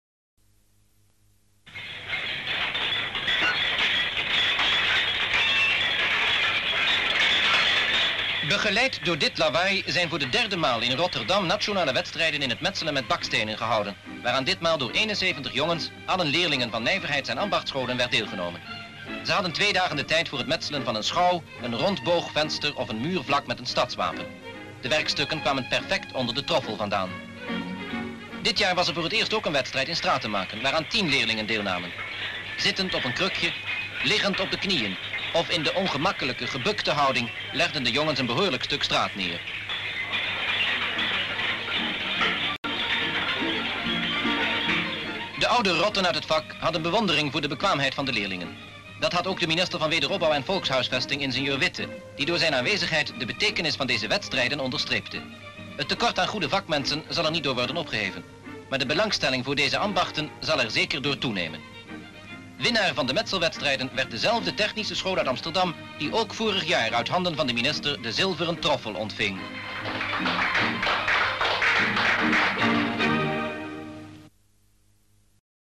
National matches bricklaying and straatmaken (Weeknummer 55-31) 🔴
Description (more): 3rd national competitions in the masonry with brick for students of industrial and ambachtscholen in Rotterdam. Making a chimney, a wall, a window with round arch with coat of arms and stratenmaken. IR. H.B.J. White, Minister of housing and reconstruction, provides the first prize, a silver trowel, to the technical School in Amsterdam.